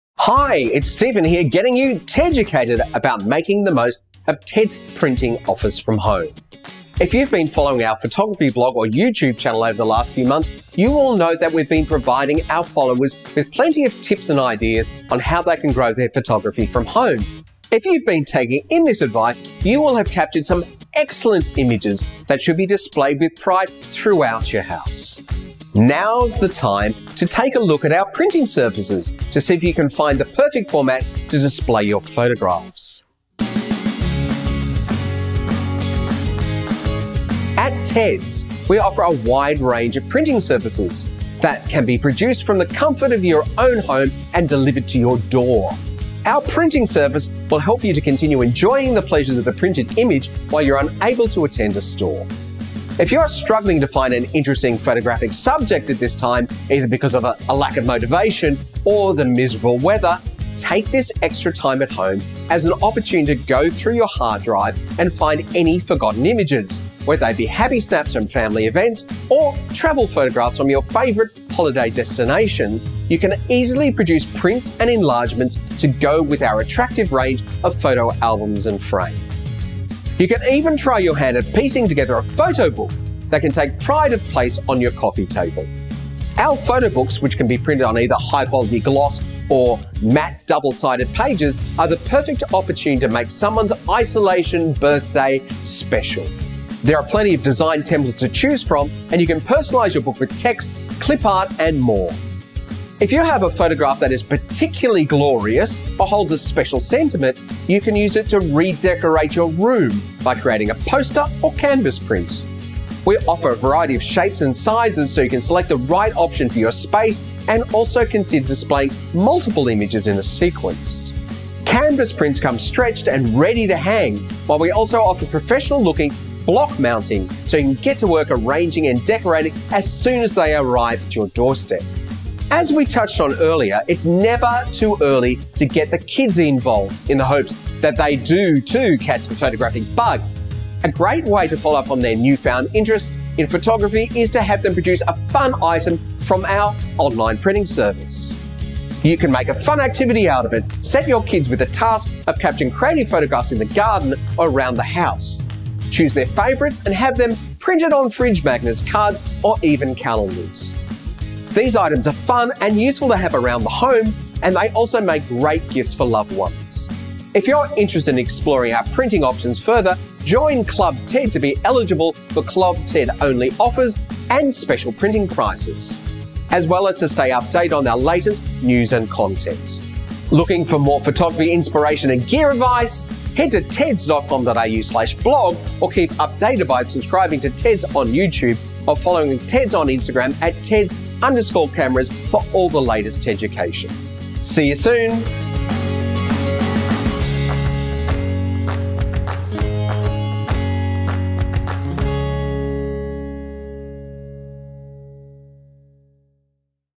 Start Printing At Home With Ted's
Ted's Academy Trainer Stephen, gets you Teducated about decorating your home with Ted's printing services.